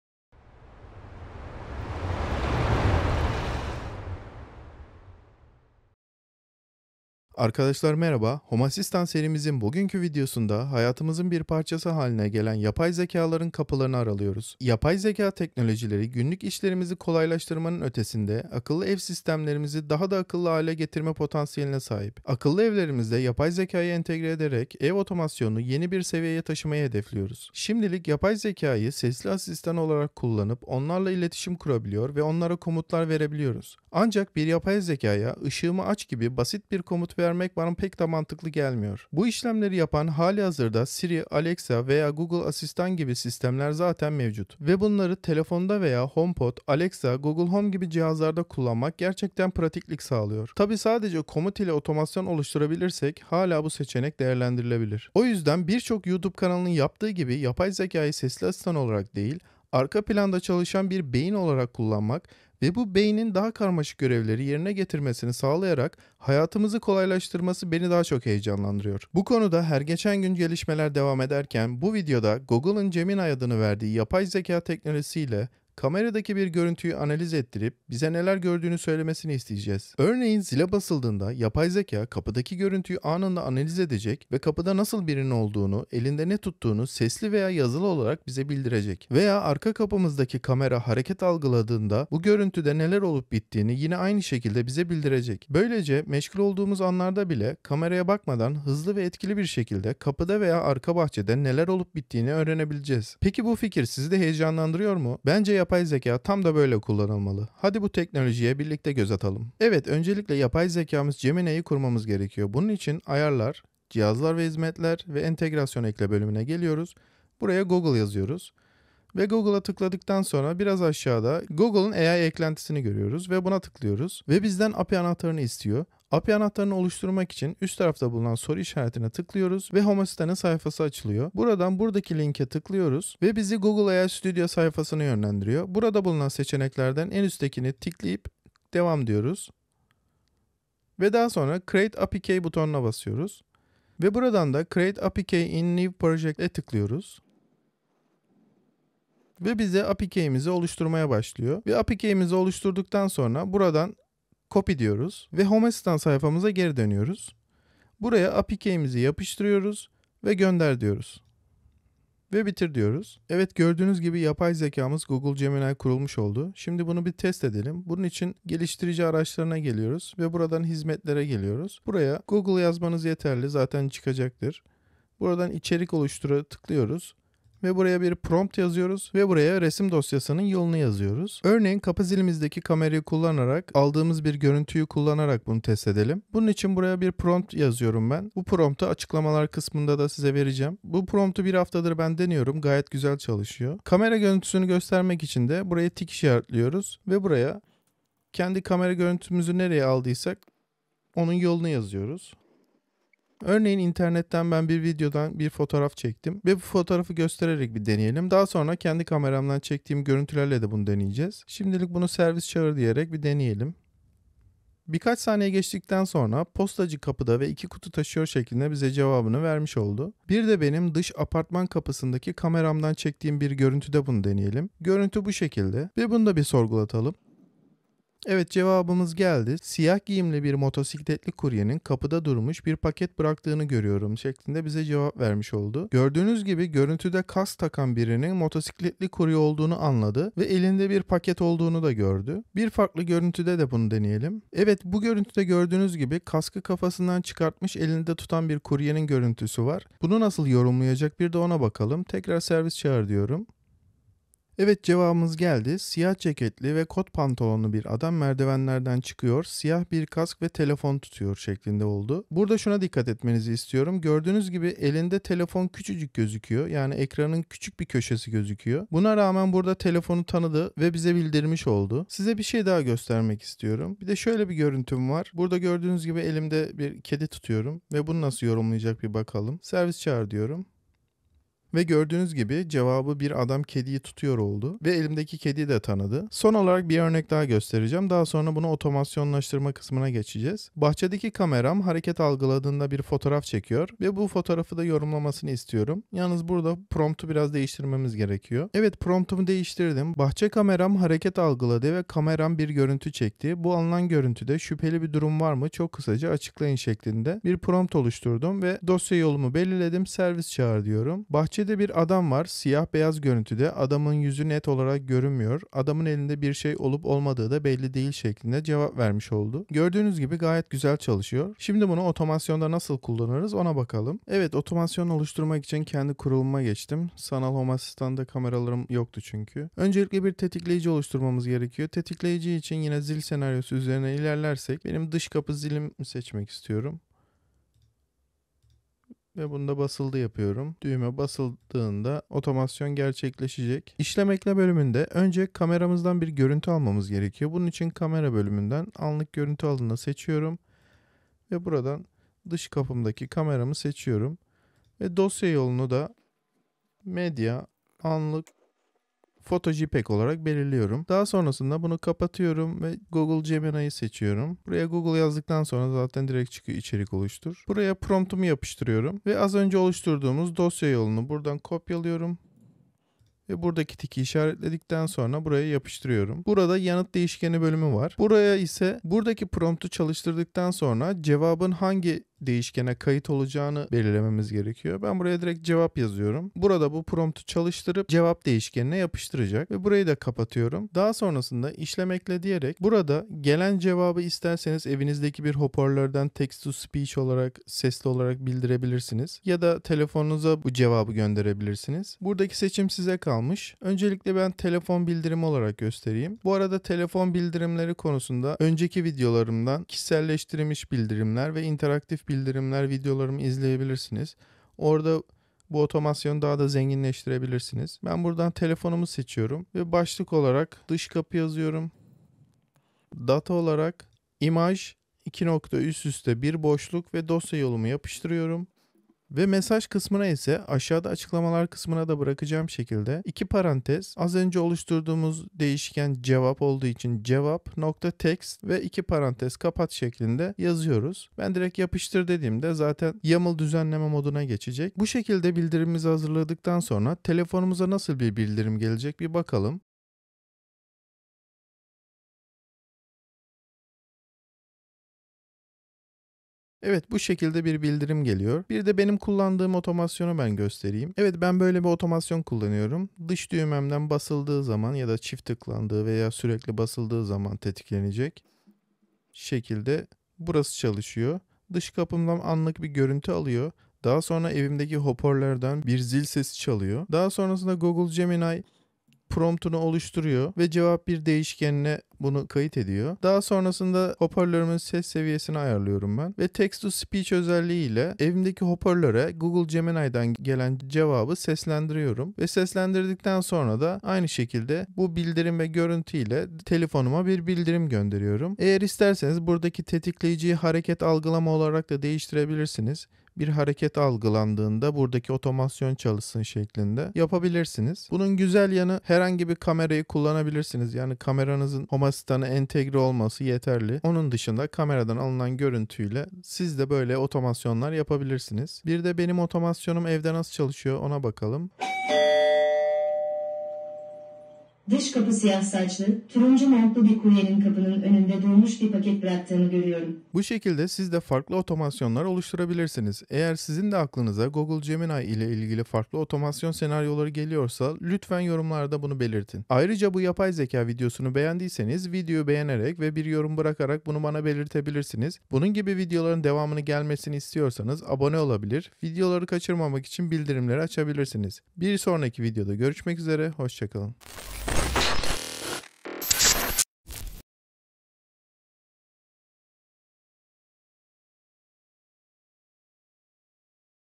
HOME ASSİSTANT - YAPAY ZEKA İLE YENİ GÖZÜNÜZ ! GOOGLE GEMINI KAMERADA KİM OLDUĞUNU BİLİYOR!
Arkadaşlar merhaba, bugün Home Assistant serimizde yapay zekaya giriş yapıyoruz.
Home Assistant'a entegre ettiğimiz en ucuz kamera bile yapay zeka ile daha akıllı hale geliyor. Ev güvenliğinde kullandığımız bu kameraları Google Gemini ile birleştirip kamerada görüntüsünde nasıl birinin olduğunu, elinde ne tuttuğunu bize yazılı veya sesli olarak  bildirecek otomasyonlar hazırlayabileceğiz. Yapay zekayı arka planda çalışan bir beyin olarak görmek sizide heyecanlandıracak. Bu arada kapak fotoğrafında ki görsel de  yapay zekaya yaptırılmıştır.

Promt:
Kapı zili kameramdan alınan bu görüntüde ne gördüğünüzü çok kısaca açıklayın. Mesajınız bir telefon bildirimine sığacak kadar kısa olmalıdır. Sabit nesneleri veya binaları tarif etmeyin.

Çoklu resim yolu vermek için:
- /media/photo.jpg
- /media/photo1.jpg
- /media/photo2.jpg

Gelen cevabı bildirim otomasyonda kullanmak için:
{{ cevap.text }}

00:00 Giriş
01:49 Google Gemini Kurulumu
02:46 Test
05:23 Otomasyon Örneği
07:39 Kendi Otomasyon Örneğim
08:50 Çıkış

Bana ulaşabilirsiniz.